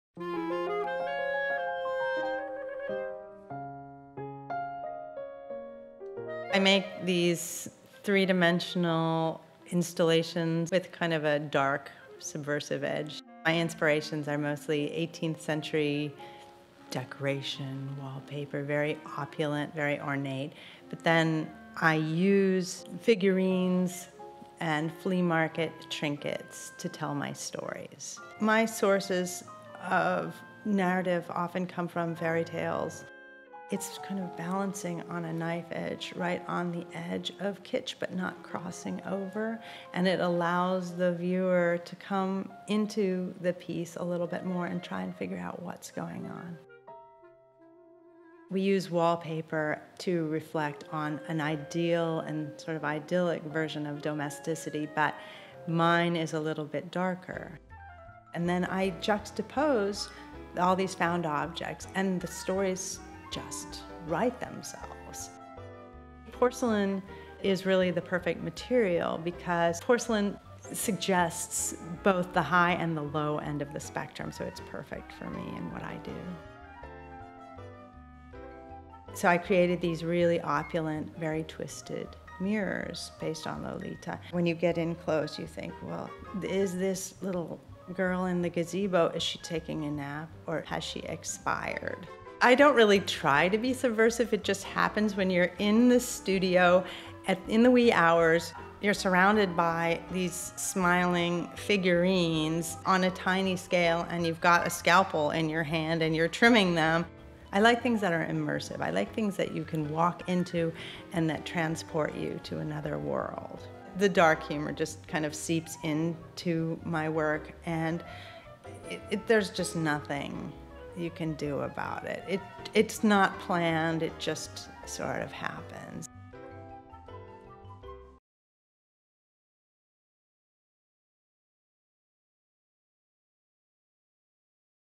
Beth Katleman Art Studio Visit
Artist Beth Katleman's creates ornate installations of 3-D porcelain wallpaper with a dark side. The figures in her pastoral scenes come from her collection of flea-market treasures.  This studio visit with Katleman takes you inside her artistic process.